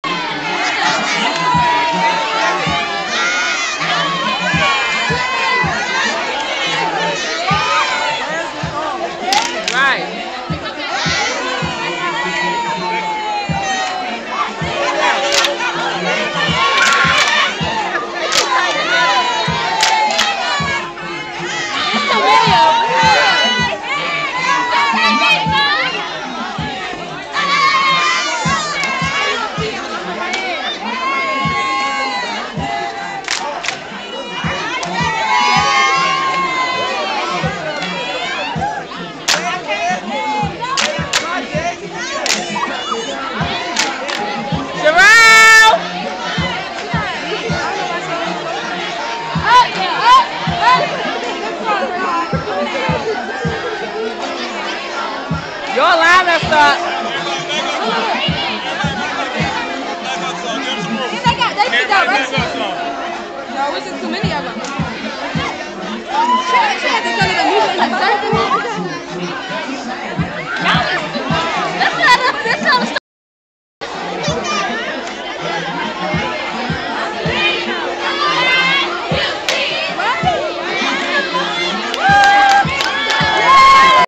Beckwith stroll team CAU 2010
beckwith dorm strolls at CAU homecome n stroll off